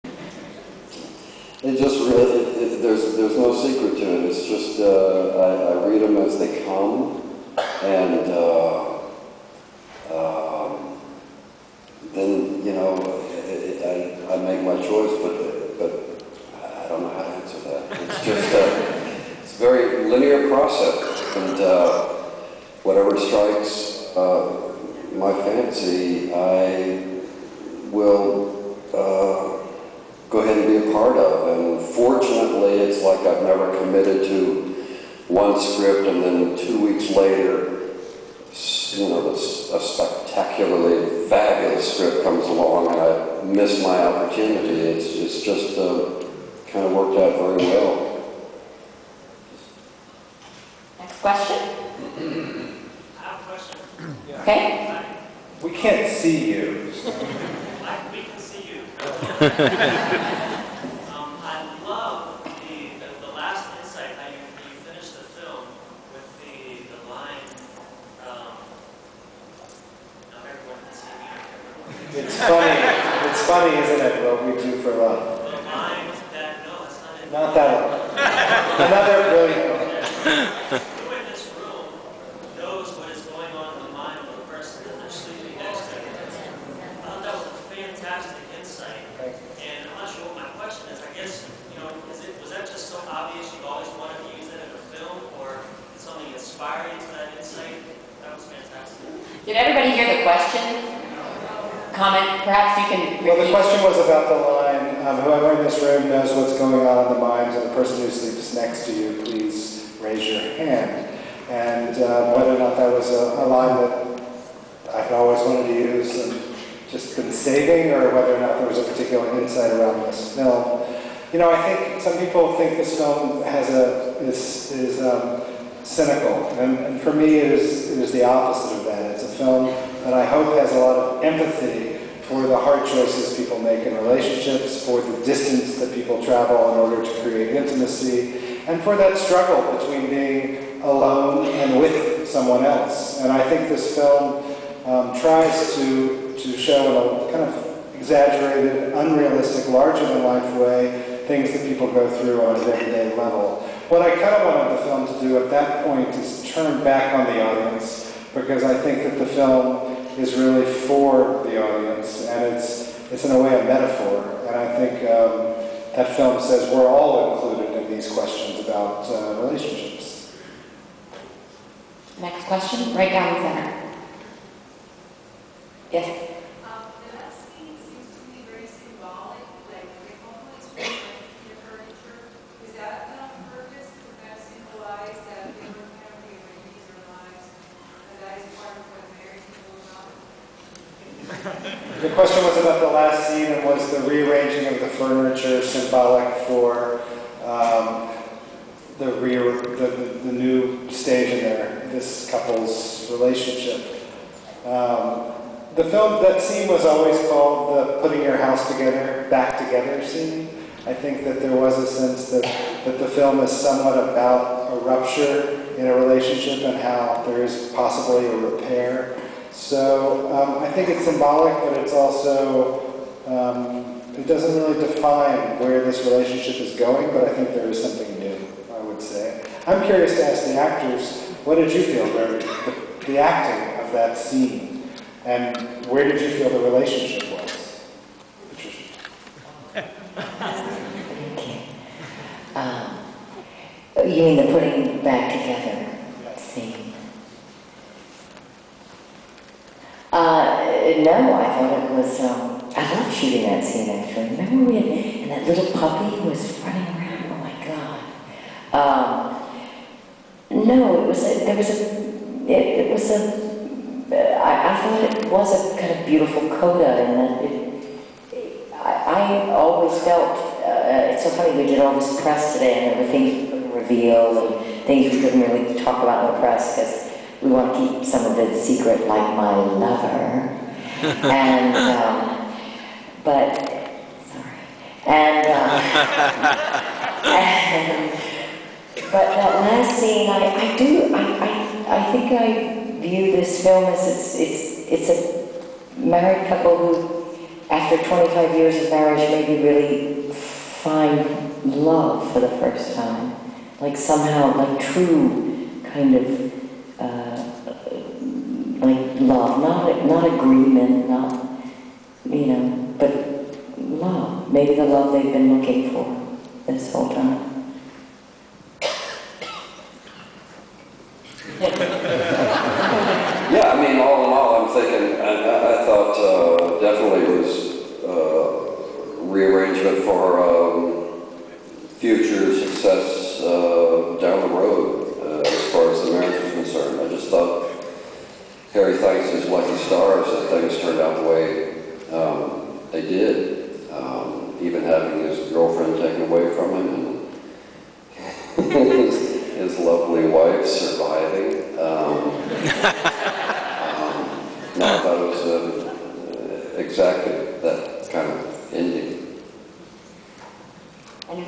MIFFClip-004
Miami International Film Festival 25th Anniversary at Gusman Theater on Friday, February 29, where Married Life director Ira Sachs and leading actors Chris Cooper and Patricia Clarkson answer audience questions following film screening.